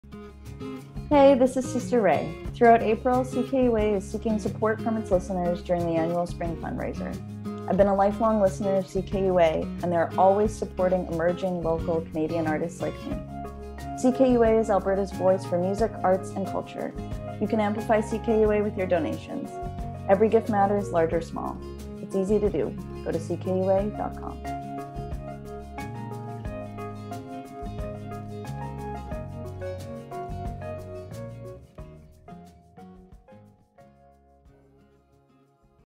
Amplify CKUA with Sister Ray!
Prior to her performance at SXSW in 2022, we caught up with Edmonton ex-pat, Sister Ray.